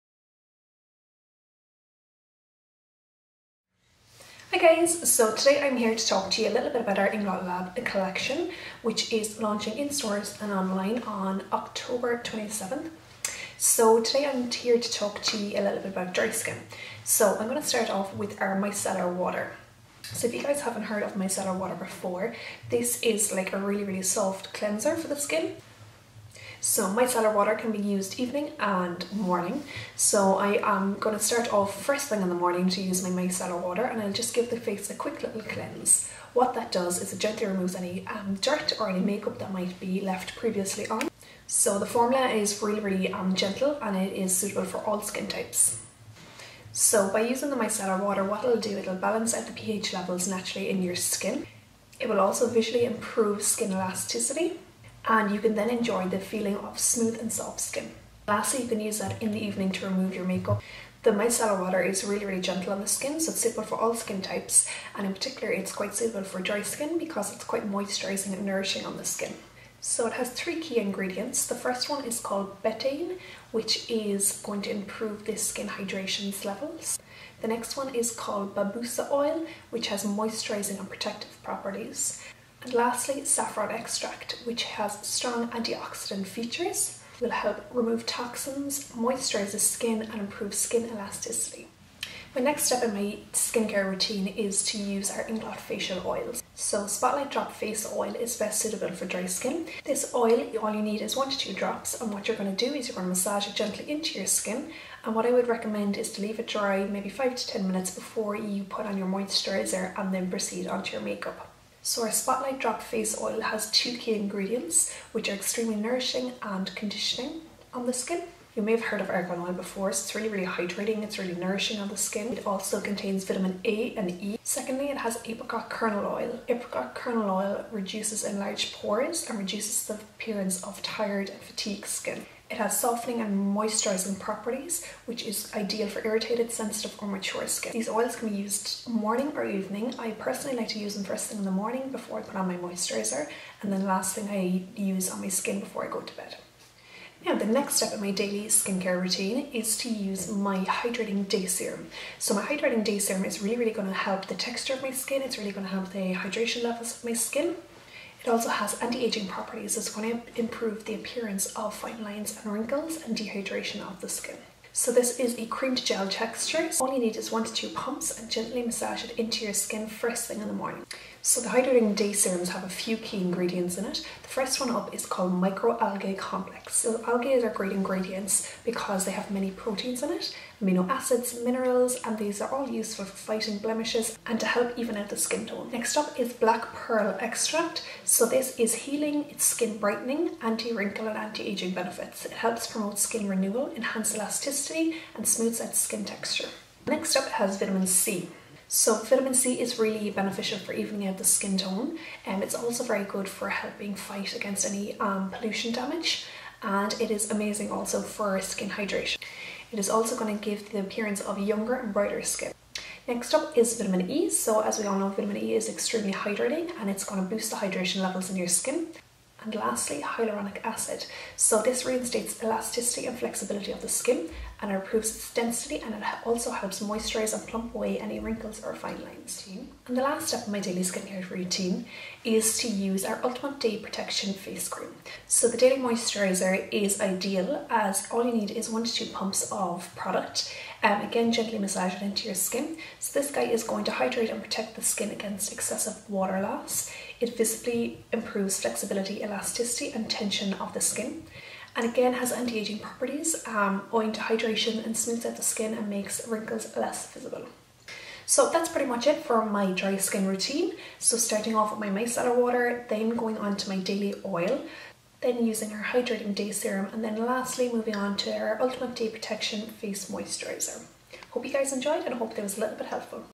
Perfect Skincare Routine for Dry Skin | Inglot Lab
Check out our Regional Trainer @sarahcumminsmua using our brand new Inglot skincare collection! ♥️
This routine is catering to people who have very dry skin!

The collection includes:

Evermatte Day Protection Day Face Cream
Inglot’s Revitilizing Night Serum
Inglot’s Hydrating Day Serum
Intense Night Recovery Face Cream
Spotlight Drop Face Oil
Sunrise Drop Face Oil
Ultimate Day Protection
Micellar Water
Dream Drop Face Oil

Let us know what you think of our new inglotskincare Collection in the comments! ♥️